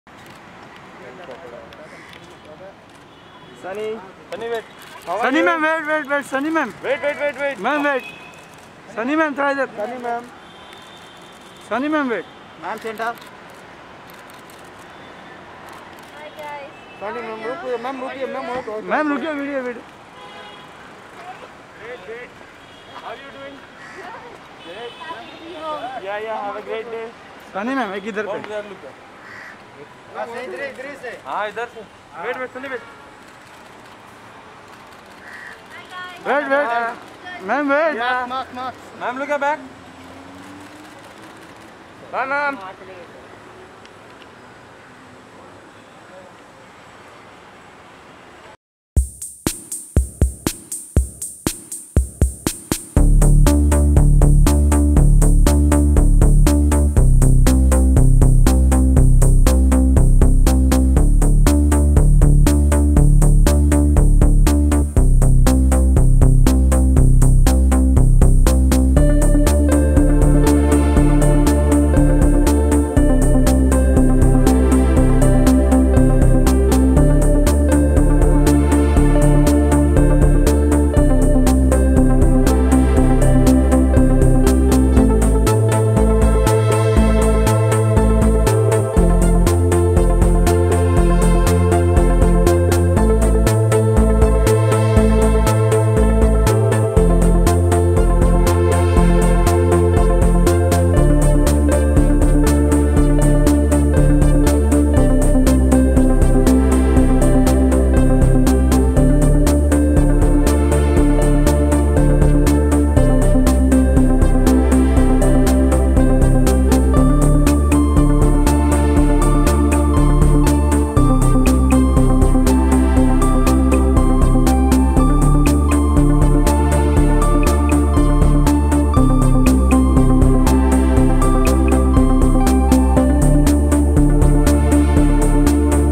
Sunny Leone in Zebra Dress Spotted Outside Saloon
WATCH SunnyLeone in Zebra Dress Spotted Outside Saloon